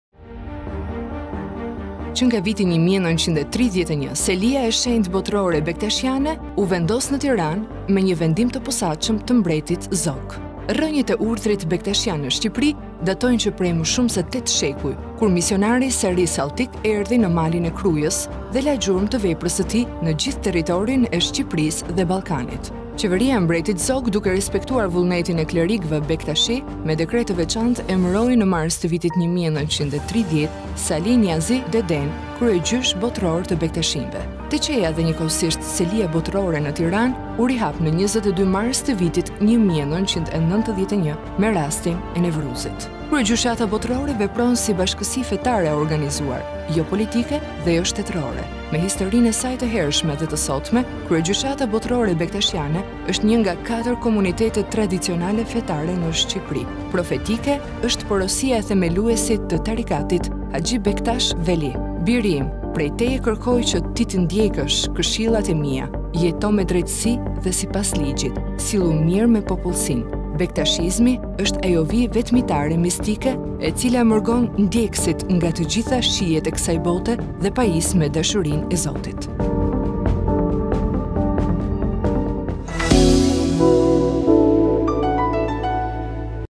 Kryegjyshata Botërore Bektashiane në Tiranë
Që nga viti 1931, me një vendim të pasaçëm të mbretit Zog   Selia e  Shënjtë Botërore  Bektashiane u vendos  në Tiranë.
Rrënjët e urdhërit bektashian (tarikat) në Shqipëri datojnë që prej më shumë se 8 shekuj, kur misionari Sari Salltik erdhi në malin e Krujës, dhe la gjurmë të veprës së tij në gjithë teritorin e Shqipërisë dhe Ballkanit.